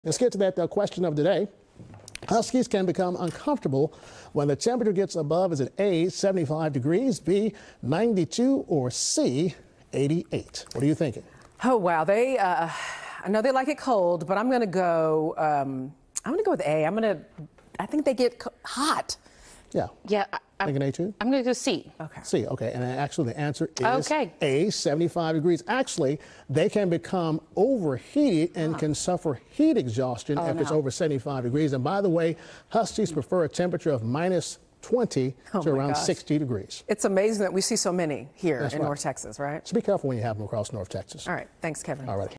Weather Quiz: Huskies and heat
NBC 5 Meteorologist Kevan Smith asks at what temperature huskies become uncomfortable.